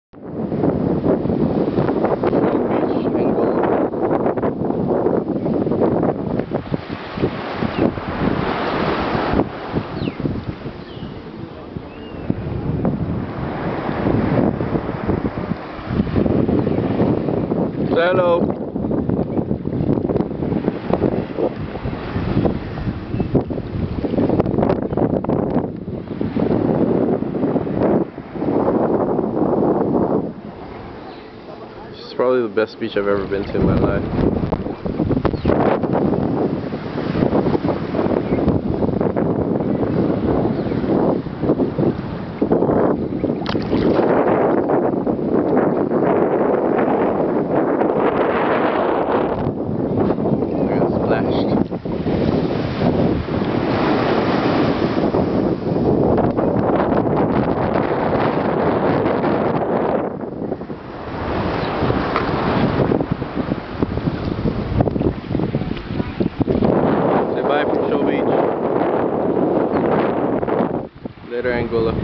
Shoal Bay Beach in Anguilla
Took the Nikon out on the water to give a glimpse of how clear the water was. Spectacular beach!